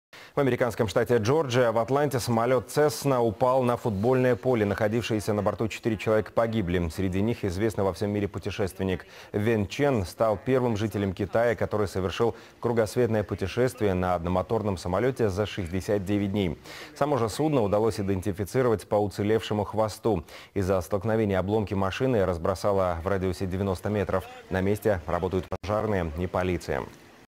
Самолет Cessna упал на футбольное поле в Атланте
Находившиеся на борту 4 человека погибли. Среди них известный во всем мире путешественник. Вен Чен стал первым жителем Китая, который совершил кругосветное путешествие на одномоторном самолете за 69 дней. Само же судно удалось идентифицировать по уцелевшему хвосту. Из-за столкновения обломки машины разбросало в радиусе 90 метров. На месте работают пожарные и полиция.